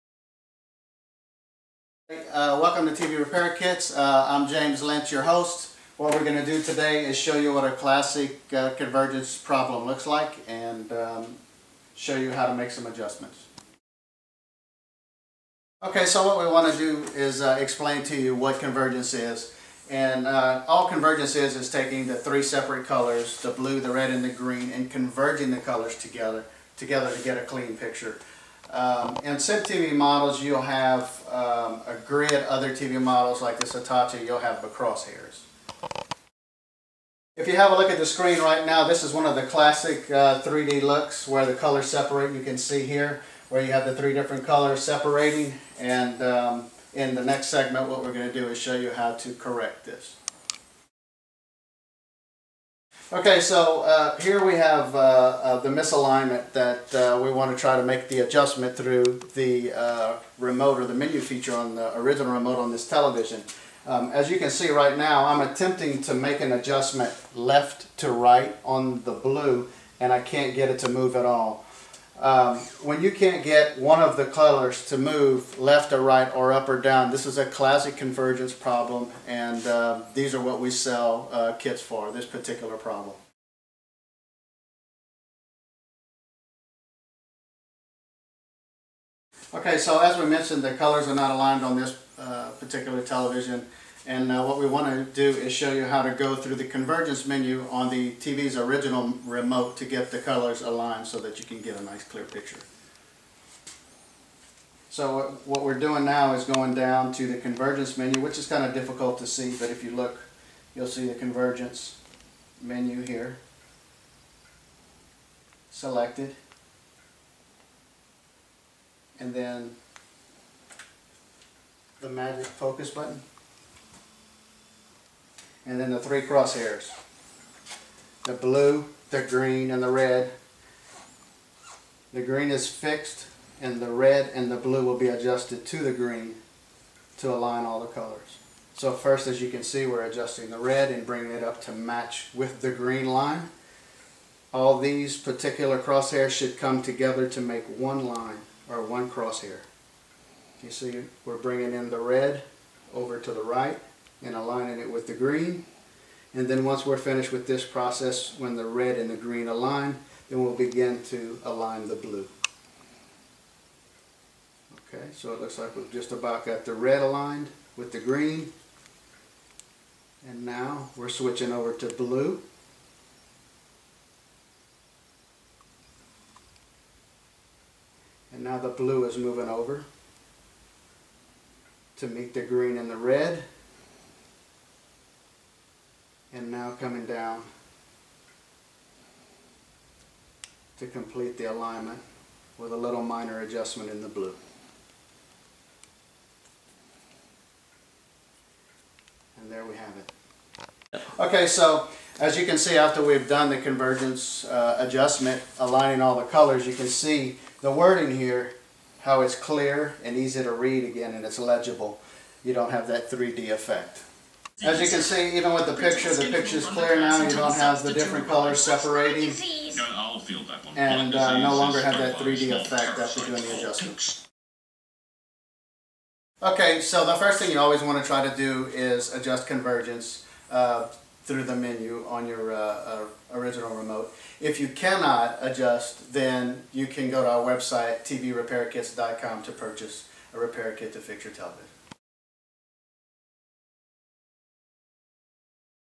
Convergence Repair Info
Projection TV Convergence Adjustment for Hitachi, Sony, Magnavox, Panasonic, Pineer, RCA, Samsung, Akai, Mitsubishi and many other rear projection TV sets.